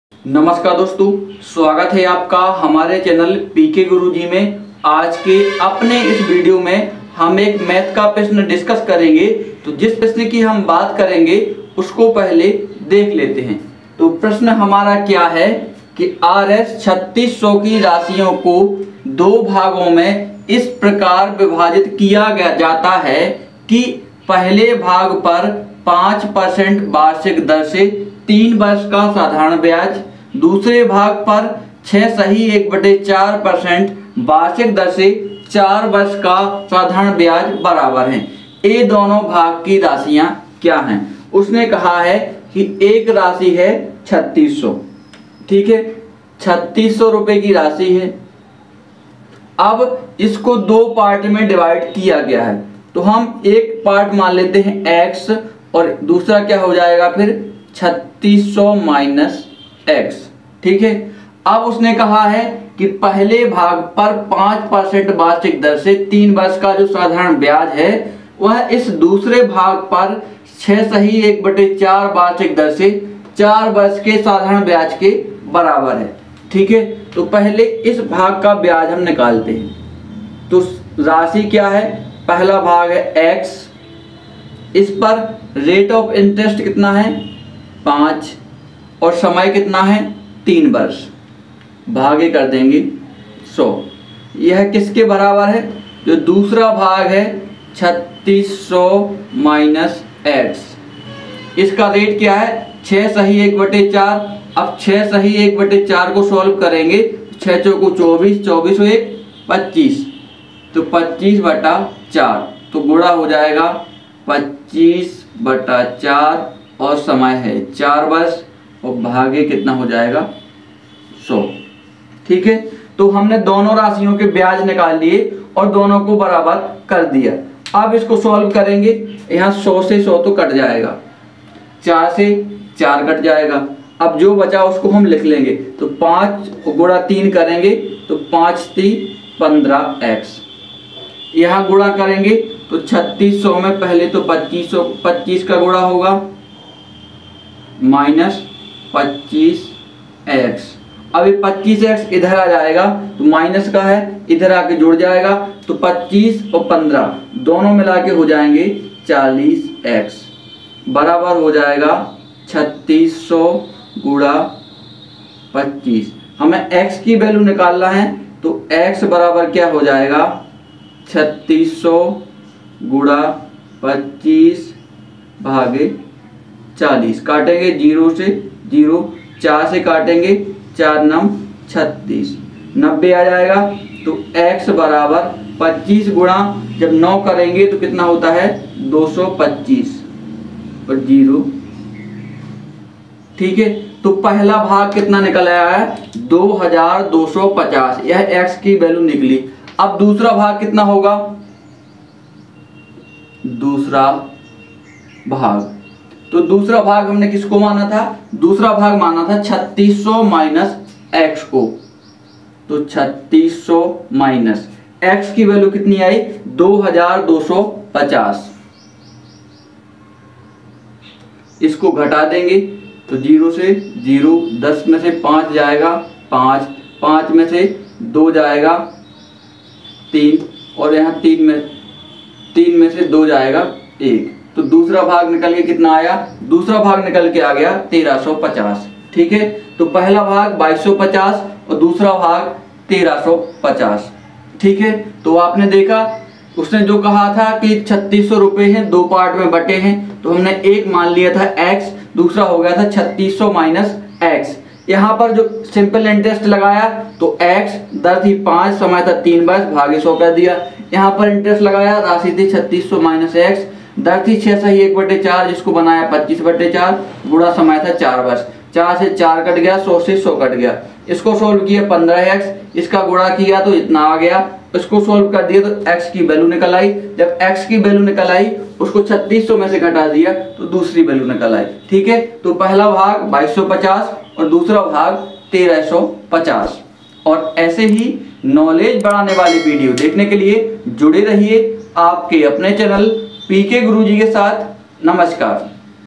Simple interest । साधारण ब्याज । S.I. Trick। Maths । Mathematics । Maths with PKGURUJee
₹3600 की राशि को दो भागों में इस प्रकार विभाजित किया जाता है कि पहले भाग पर 5% वार्षिक दर से 3 वर्ष का साधारण ब्याज, दूसरे भाग पर 6 1/4 प्रतिशत वार्षिक दर से 4 वर्ष के साधारण ब्याज के बराबर हैं । यह दोनों भाग की राशियां क्या हैं?

A sum of ₹ 3600 is divided into two parts in such a way that the simple interest on the first part at 5% per annum for 3 years is equal to the simple interest on the second part at 6 1/4 per cent per annum for 4 years. What are the sums of these two parts?

₹3600 kee raashi ko do bhaagon mein is prakaar vibhaajit kiya jaata hai ki pahale bhaag par 5% vaarshik dar se 3 varsh ka saadhaaran byaaj, doosare bhaag par 6 1/4 pratishat vaarshik dar se 4 varsh ke saadhaaran byaaj ke baraabar hain . yah donon bhaag kee raashiyaan kya hain?